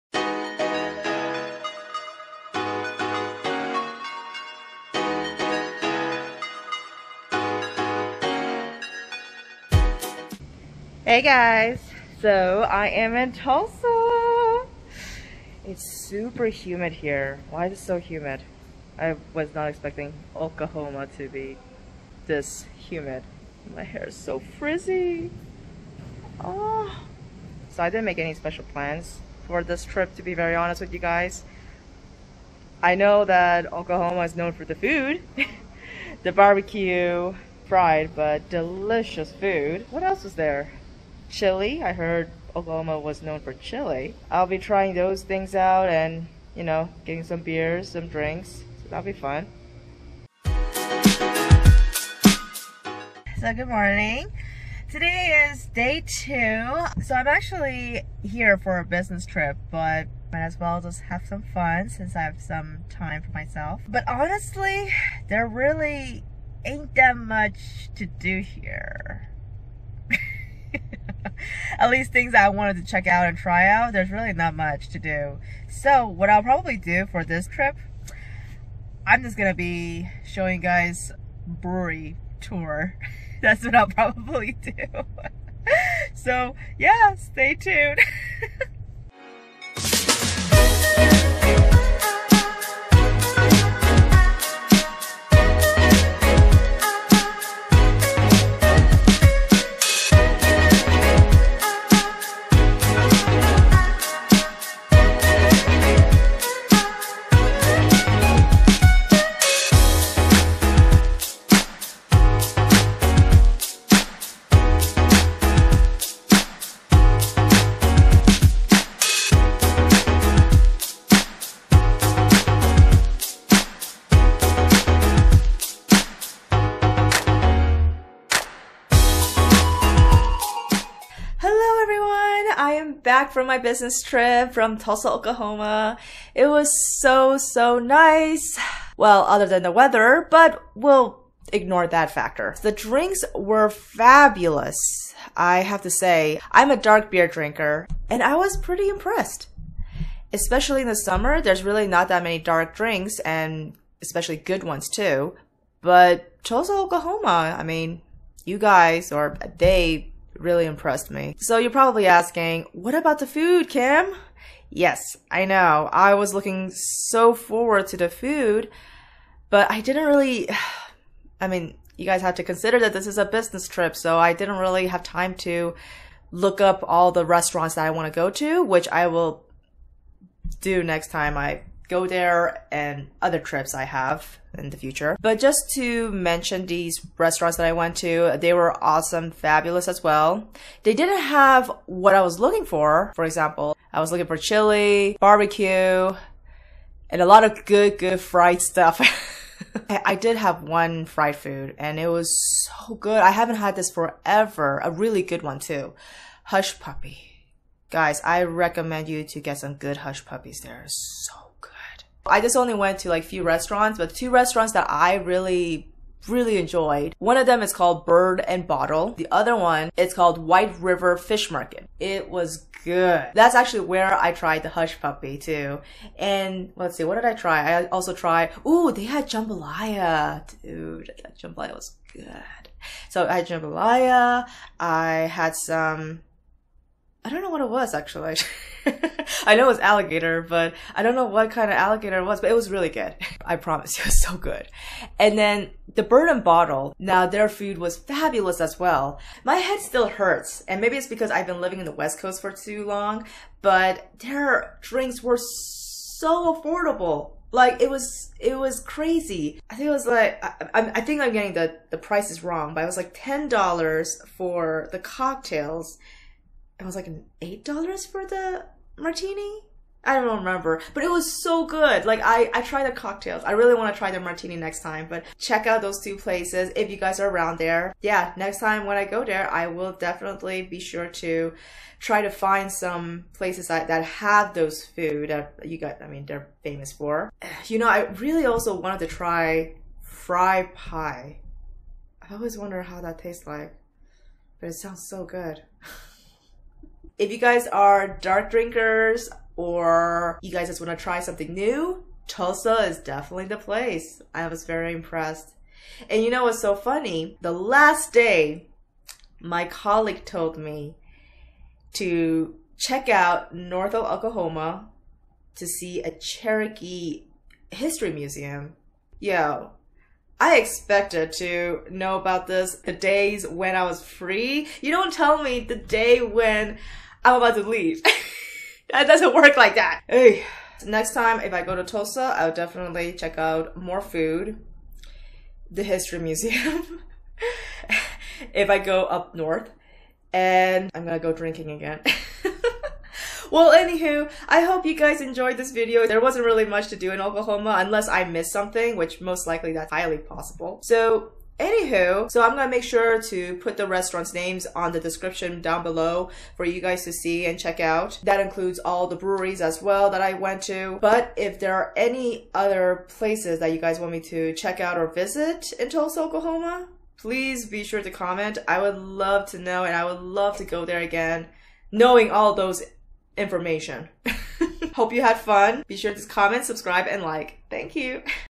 Tulsa, OK Trip of Brewery Hops and Good Food
This video includes visiting:

Tulsa, OK

List of Breweries:
Nothing's Left Brewing Co.
Cabin Boys Brewery
American Solera
Renaissance Brewing Co.
Broken Arrow Brewing Co.
Nook Brew Co.

List of Restaurants:
Bird and Bottle
White River Fish Market